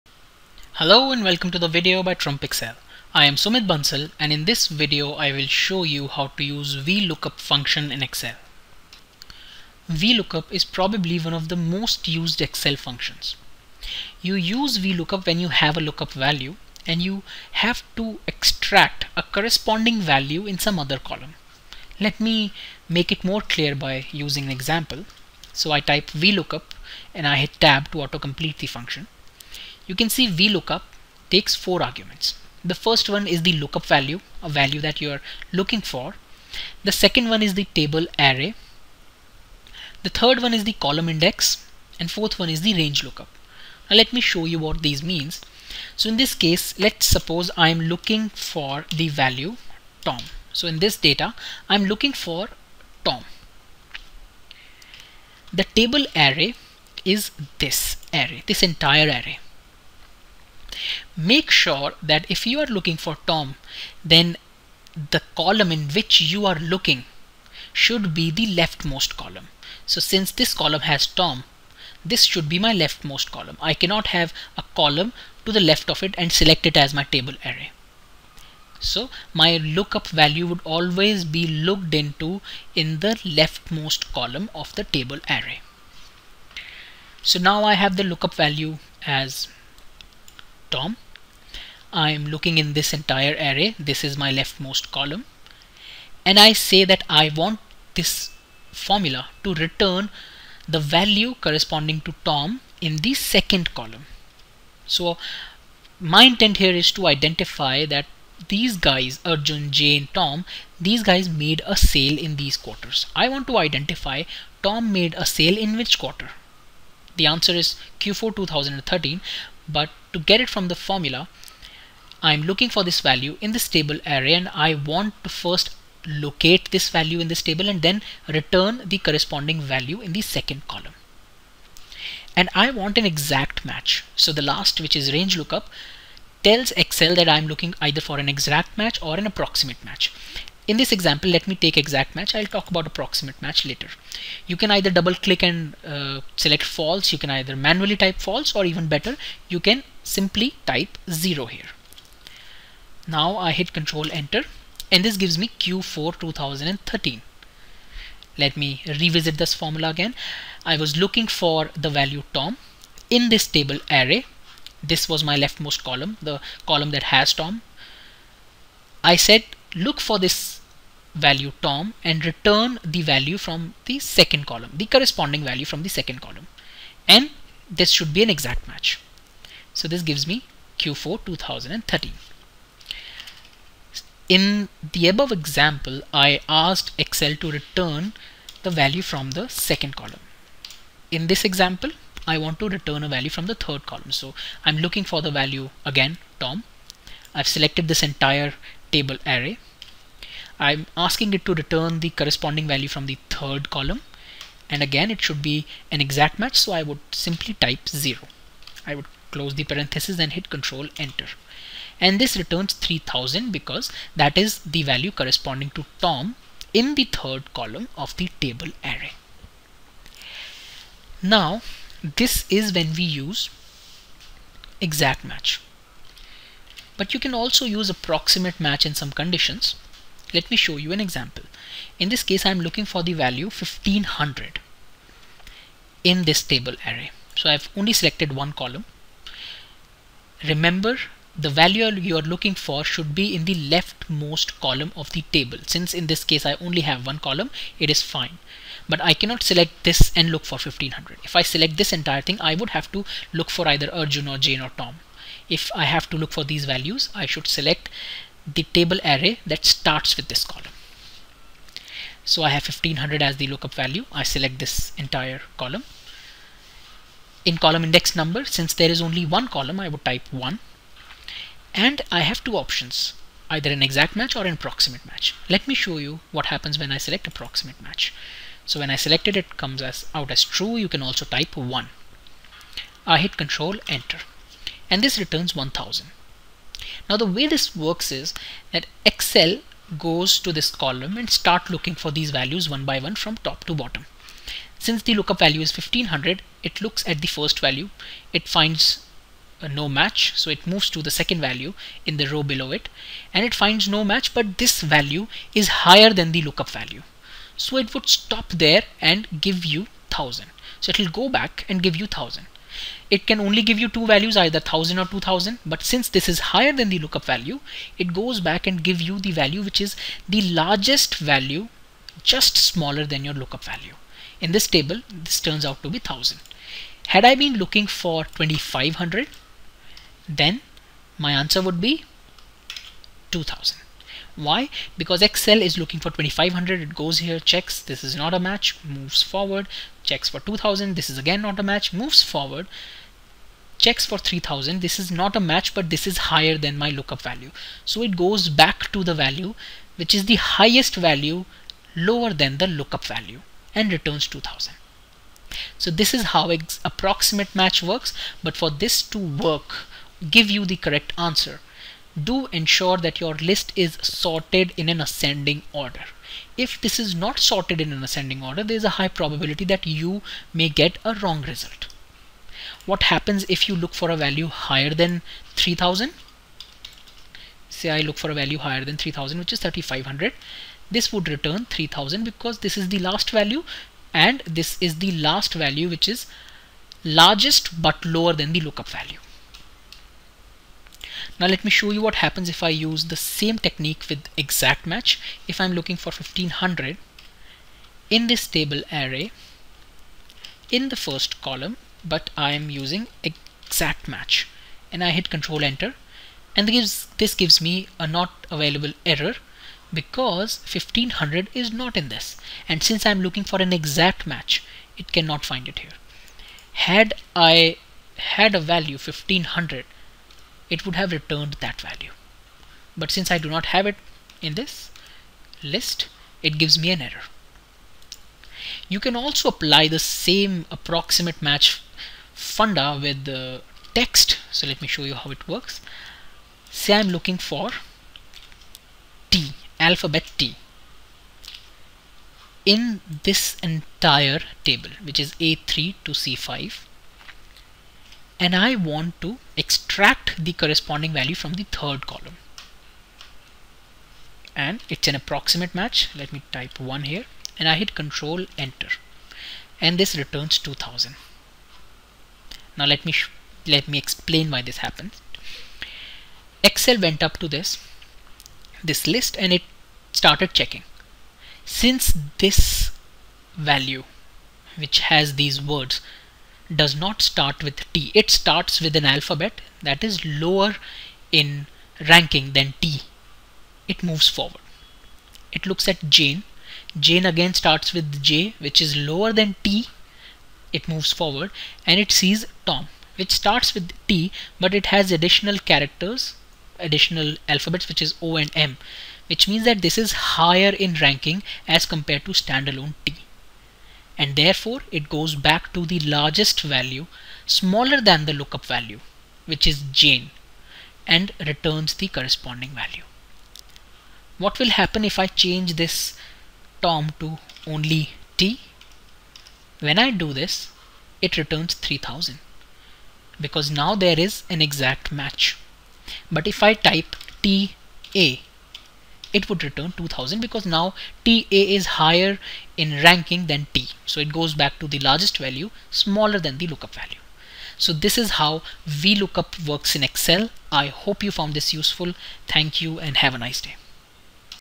VLOOKUP Formula in Excel
VLOOKUP formula looks for a given value in a vertical list, and once it has spotted that value, it would use that row and return the value from the specified column number